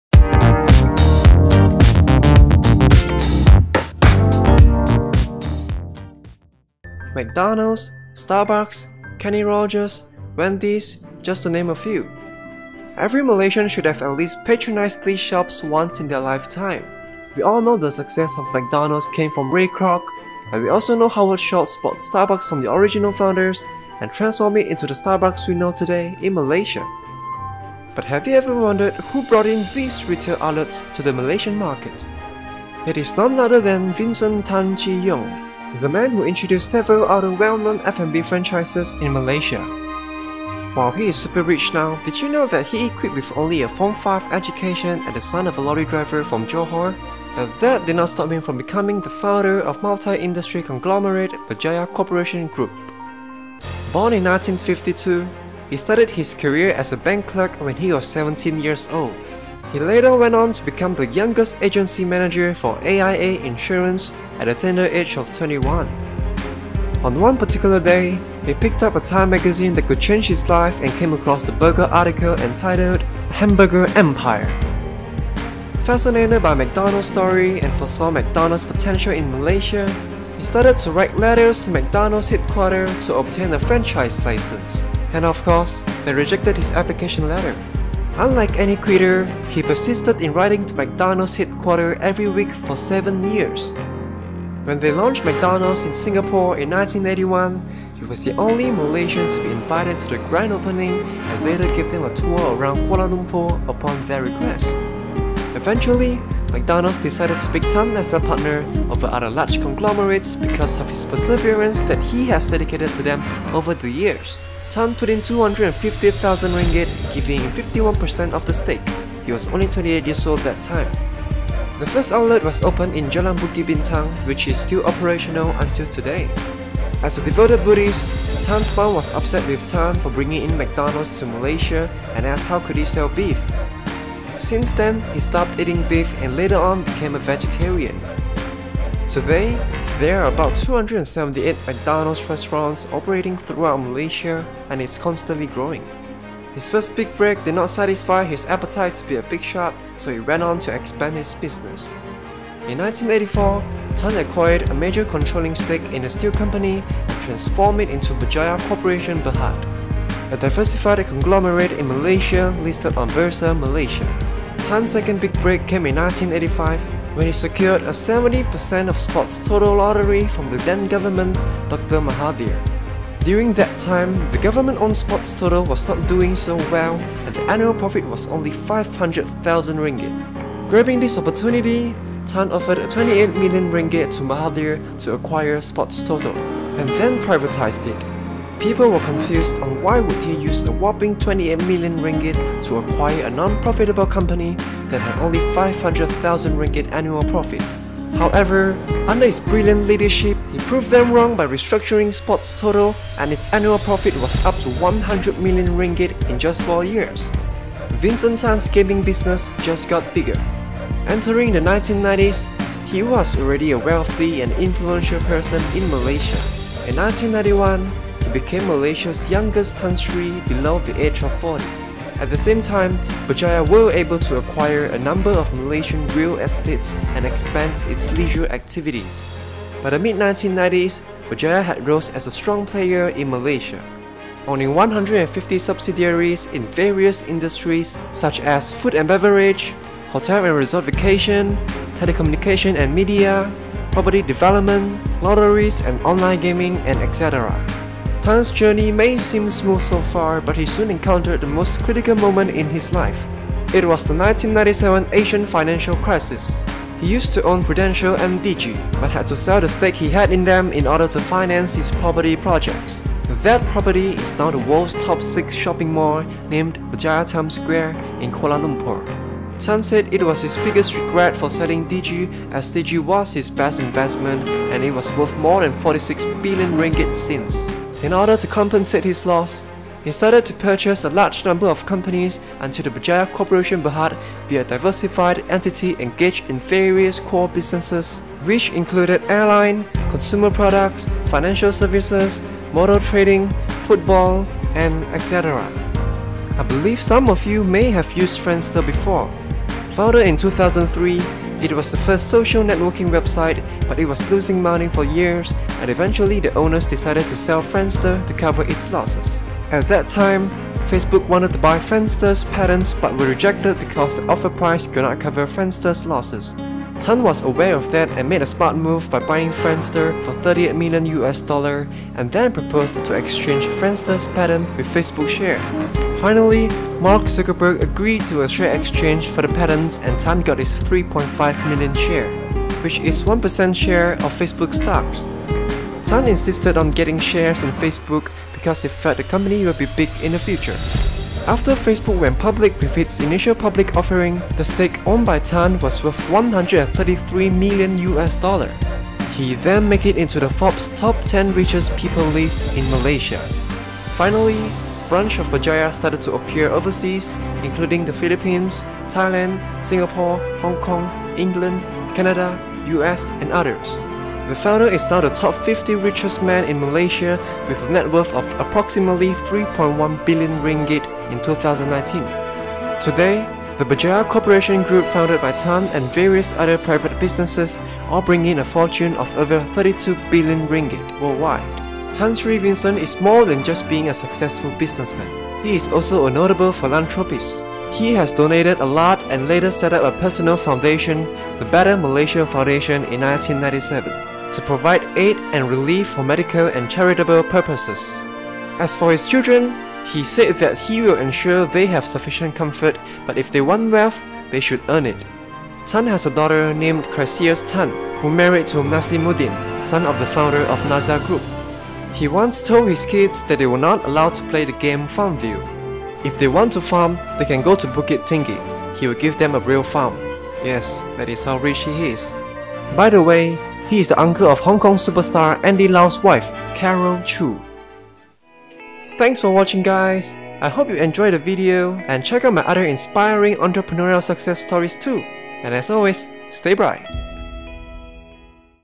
【Vincent Tan】Just a Form 5 boy ♦ How Mahathir turns him from a Millionaire into a Billionaire
• 1952 – Born in Batu Pahat, Johor.
• 1973 – Become the youngest agency manager for AIA Insurance.
• 1981 – He was the only Malaysian to be invited to the grand opening of McDonald’s in Singapore
• 1982 – The first outlet of McDonald’s was opened in Jalan Bukit Bintang, Malaysia.
• 1984 – He acquired a major controlling stake in a steel company and transformed it into Berjaya Corporation Berhad.
• 1985 –  He secured a 70% of Sports Toto lottery from Mahathir.
• 1991 – He became Malaysia’s youngest Tan Sri below the age of 40.
• 1997 –He used to own Prudential and Digi, but had to sell the stake he had in them.
• 2008 – Bought Friendster and sold its patent to Facebook.
• 2019 – The founder is now the Top 50 richest man in Malaysia with a net worth of approximately RM3.1bil.

The Berjaya Group is currently involved in diverse businesses such as consumer marketing, multi-level direct selling, hotels and resorts, recreational development, property investment and development, financial services, food and beverage, telcos and media, education, motor vehicle distribution, gaming and lottery management, football clubs and the environmental sanitary landfill business.

Videos credIt to: videvo